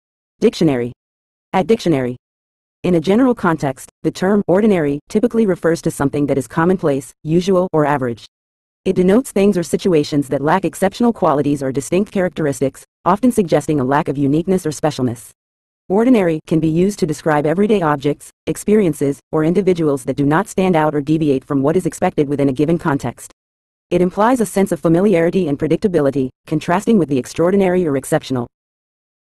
Ordinary Meaning In English
In a general context, the term "ordinary" typically refers to something that is commonplace, usual, or average. It denotes things or situations that lack exceptional qualities or distinct characteristics, often suggesting a lack of uniqueness or specialness. "Ordinary" can be used to describe everyday objects, experiences, or individuals that do not stand out or deviate from what is expected within a given context. It implies a sense of familiarity and predictability, contrasting with the extraordinary or exceptional.